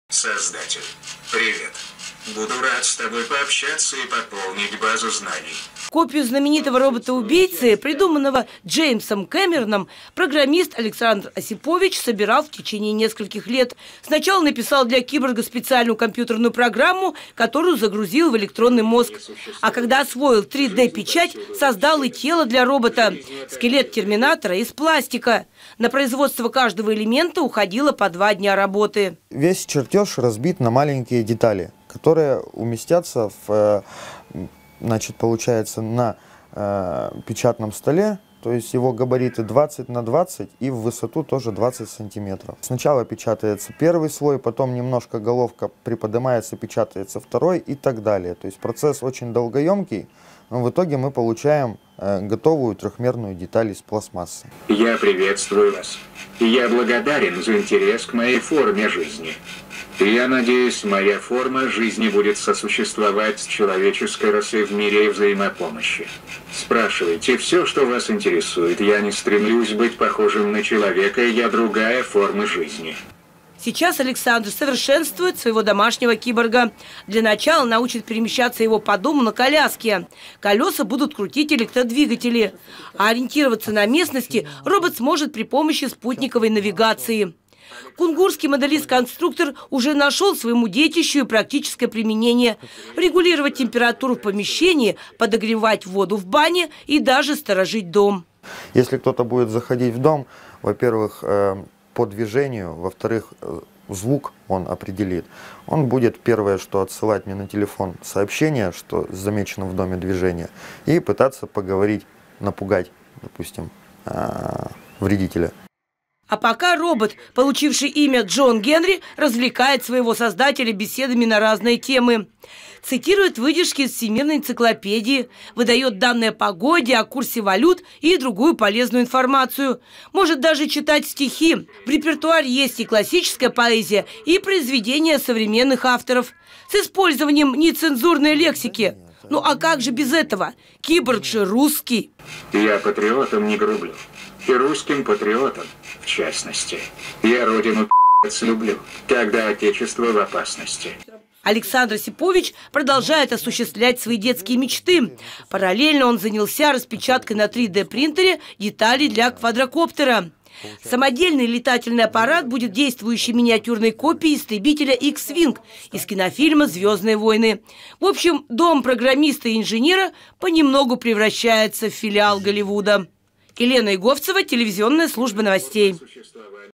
В Прикамье программист собрал у себя дома копию Терминатора Т-800
Кунгурский филиал Голливуда. Мы уже рассказывали, что прикамский программист Александр Осипович распечатал на 3D-принтере и собрал у себя дома модель Терминатора Т-800. Робот известен по фильмам Джеймса Кэмерона. Машина умеет двигать руками и головой, поддерживает беседу и накапливает информацию. Копию героя одноименного голливудского блокбастера воочию увидела Елена Еговцева.